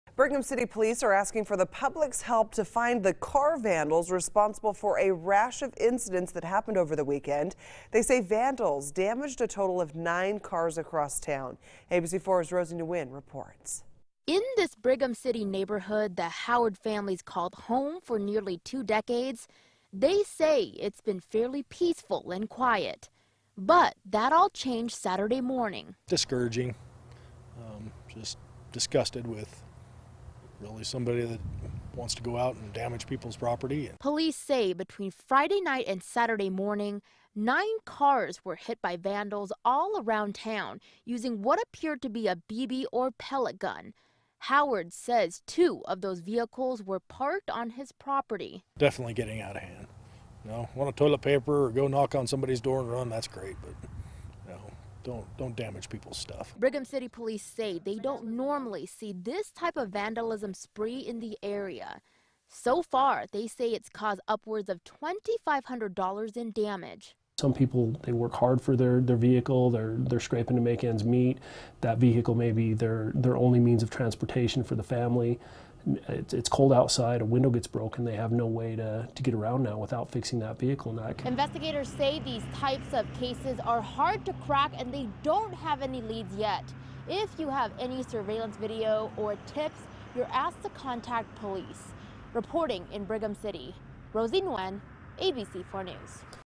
Brigham City Police searching for suspects in overnight vandalism spree (5 p.m.)
Brigham City Police is asking for the public's help in finding the people responsible for an overnight vandalism spree that damaged nine vehicles all around town over the weekend.